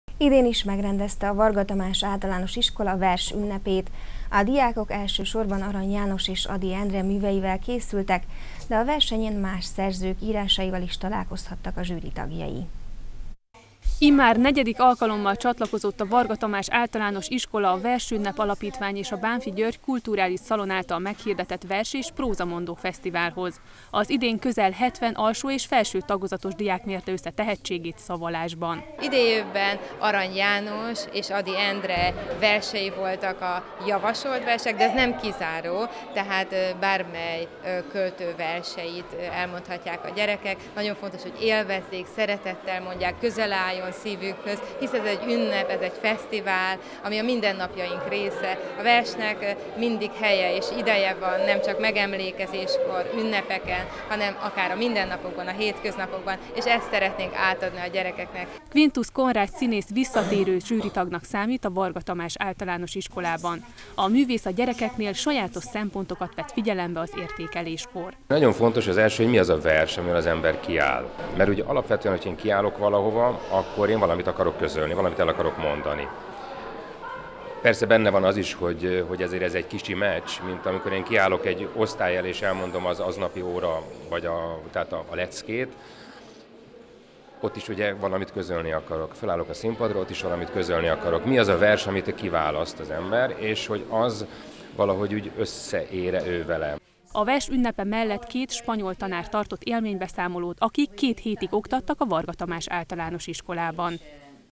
Közel hetven diák szavalt a Vers ünnepén
Idén is megrendezte a Varga Tamás Általános Iskola a Vers ünnepét. A diákok elsősorban Arany János és Ady Endre verseivel készültek, de a versenyen más szerzők műveivel is találkozhattak a zsűri tagjai.